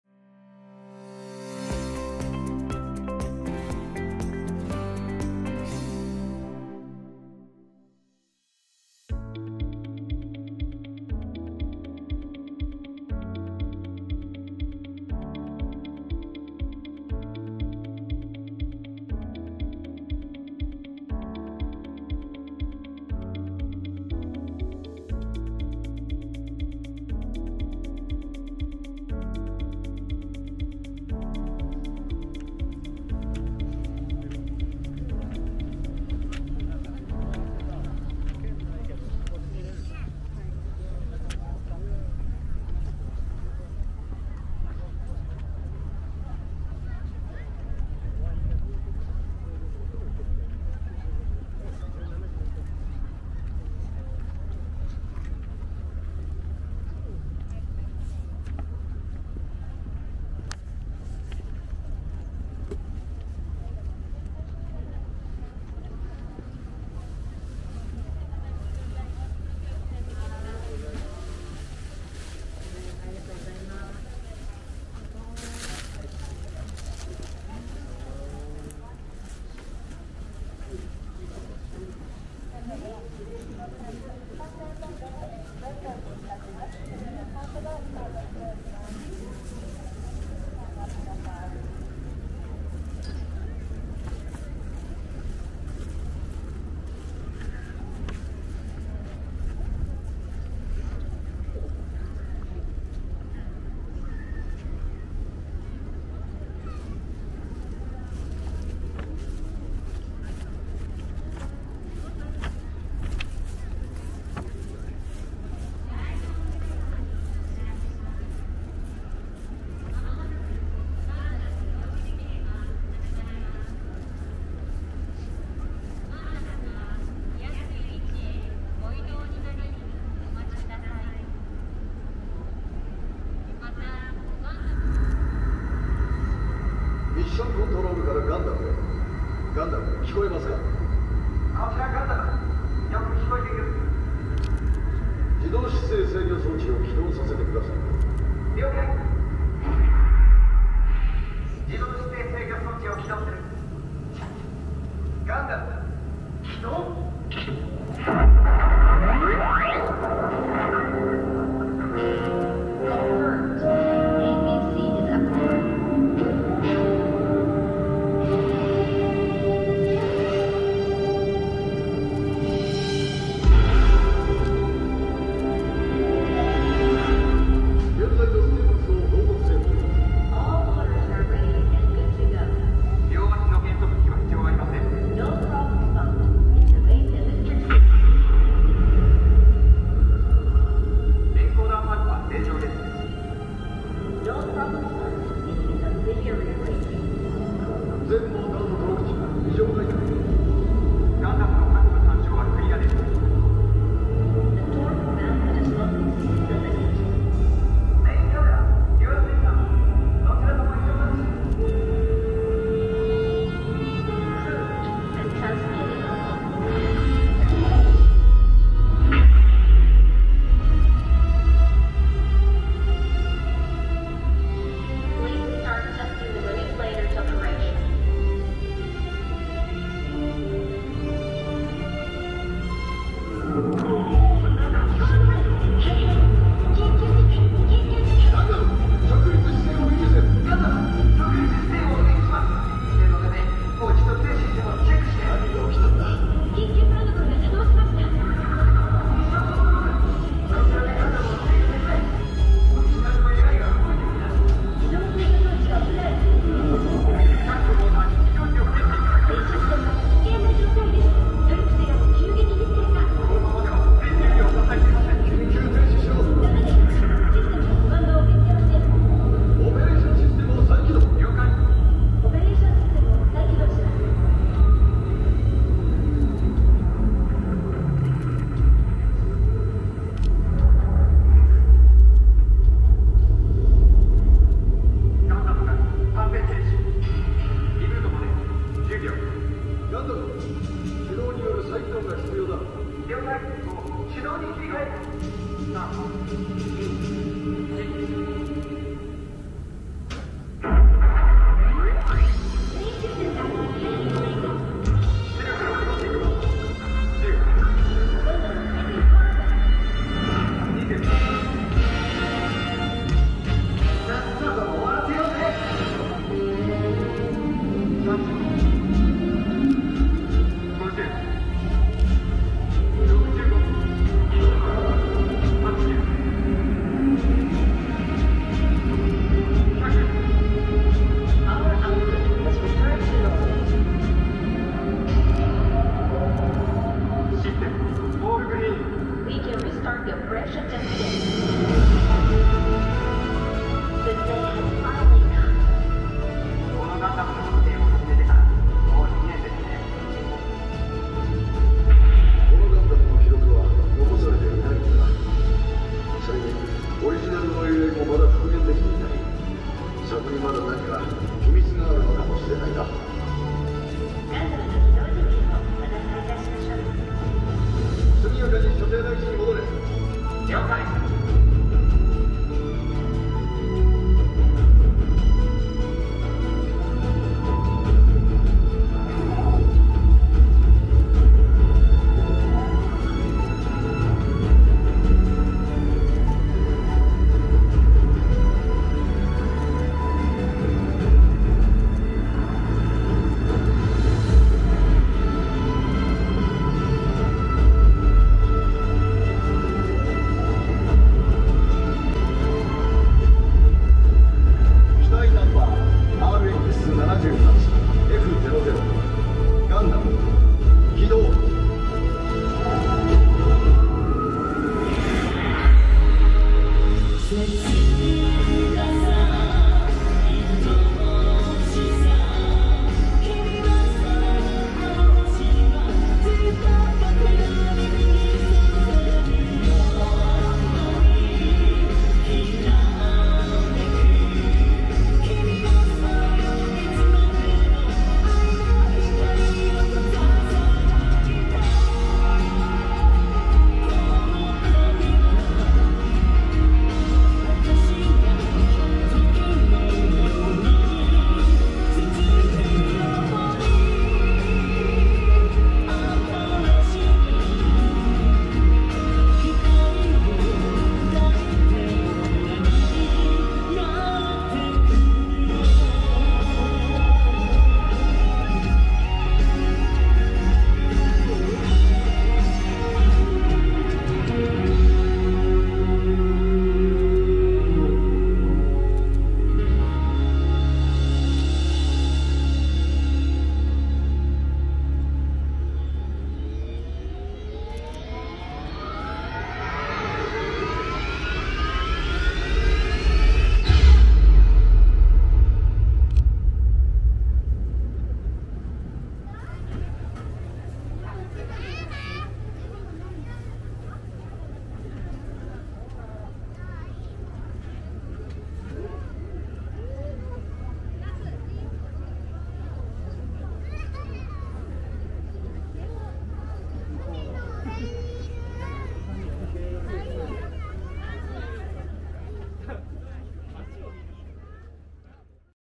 2021 Full-Scale Moving GUNDAM Show | Yokohama Walking Tour
Wouldn't you like to see a show of a giant moving Gundam illuminated by a beautiful sunset?

In addition to the show, the Gundam Factory Yokohama has a Gundam-themed cafe, a store where you can buy Gundam plastic models, and an exhibition room where you can learn about Gundam.
This event will be held until March 31, 2022, so you'd better hurry if you want to see it.